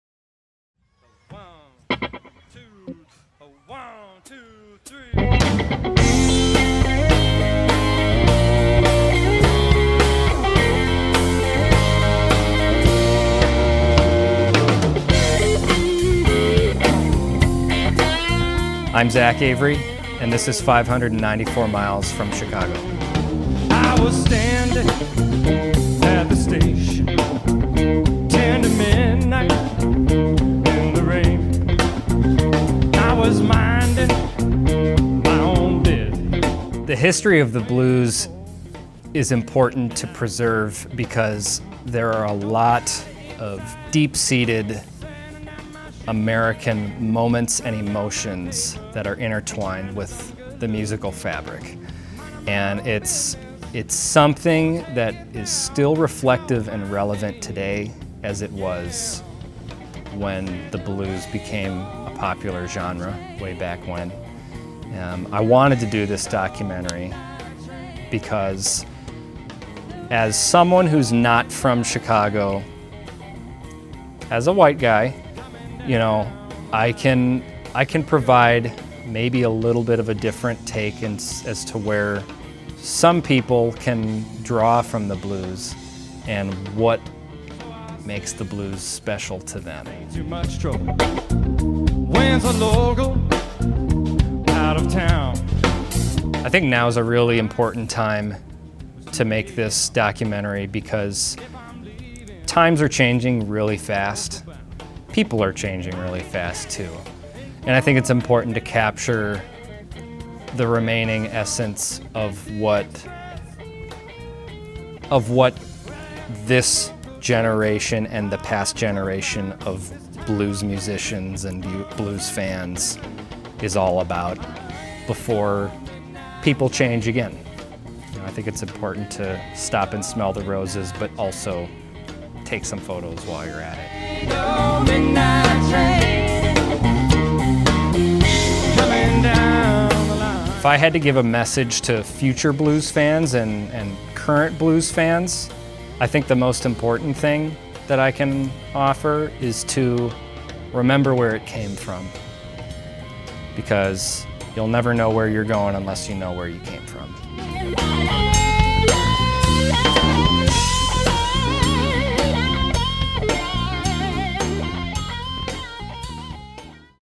594 MFC: 3 Questions w/ Zach Avery
This is part of our new short video series for 594 Miles From Chicago, called "3 Questions with". Each artist we interview will have 3 questions to answer at the end of their interview! In this video we have Zach Avery who joins us at Buddy Guy's Legends in Chicago, IL.

About Zach Avery:
Zach Avery began playing in bars and venues at the age of 13. The Minnesota native was active in the Twin Cities’ local music scene, and began sitting in with bands and open jams throughout the area. In January of 2014 and in 2015, he helped lead two different bands down to Memphis, TN to partake in the International Blues Challenge. He moved to Chicago, IL in 2019 to study at Columbia College and quickly established roots in the local Blues and Rock scene. He formed a band called “The Smokin’ Tones” (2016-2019) which played frequent shows around the Chicago area. In 2019 he formed The Zach Avery Band and continues to play Blues, Rock and Soul regularly around the Chicago area. As the lead singer and guitarist, his music is arranged to move the body and soul, while incorporating a guitar-driven, tight groove.